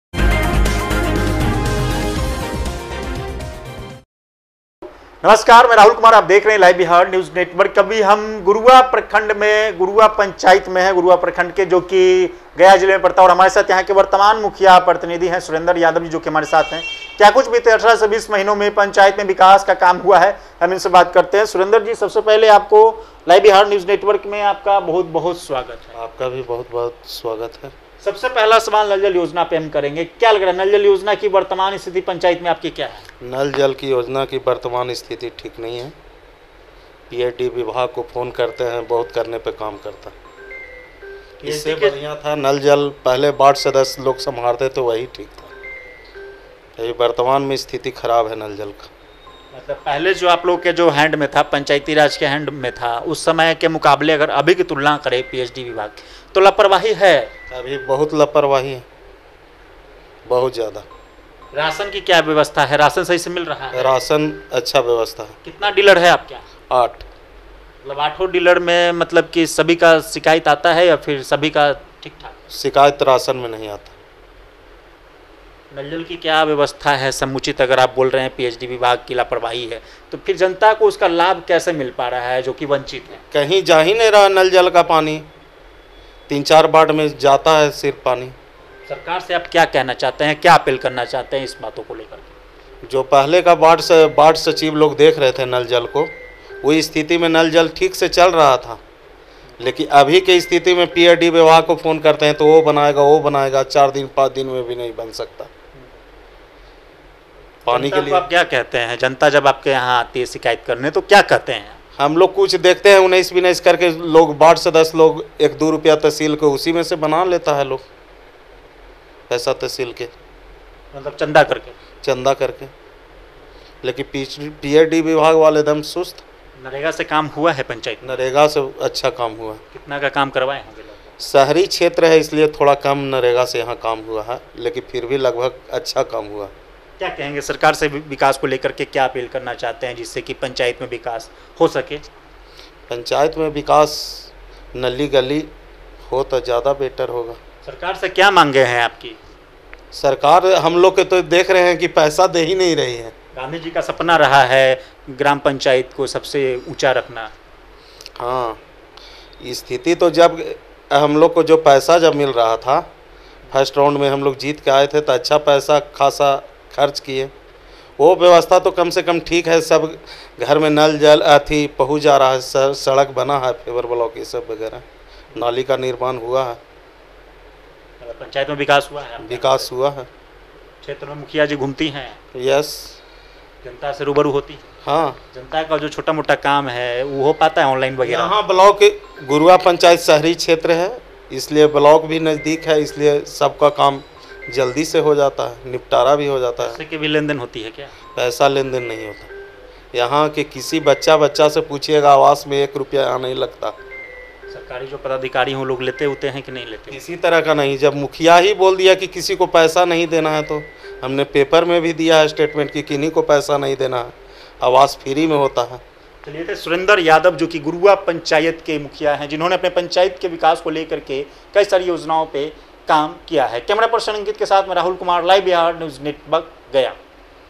गुरुवा पंचायत के मुखिया कार्यवाहक सुरेंद्र यादव से पंचायत पर खास बातचीत
हमारा_लक्ष्य_स्वस्थ_भारत दूसरा चैनल Live Bihar Jharkhand Network